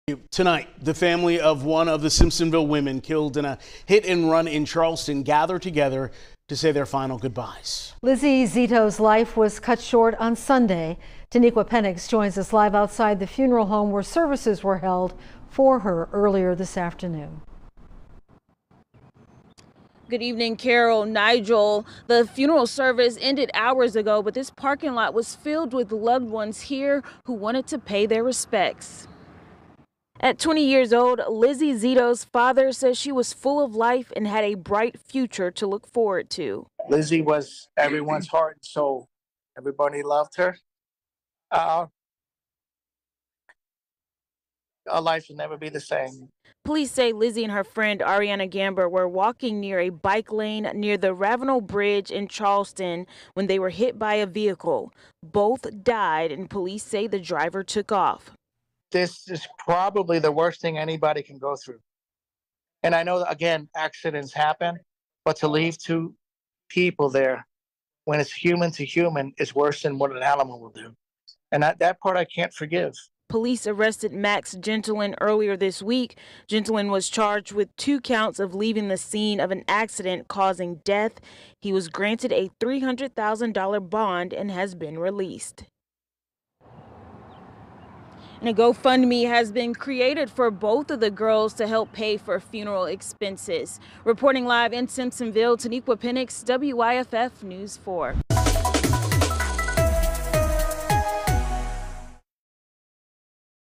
Funeral services held for Simpsonville, South Carolina, woman killed in hit-and-run in Charleston
The family of one of the Simpsonville women killed in a hit-and-run in Charleston gathered together to say their final goodbyes.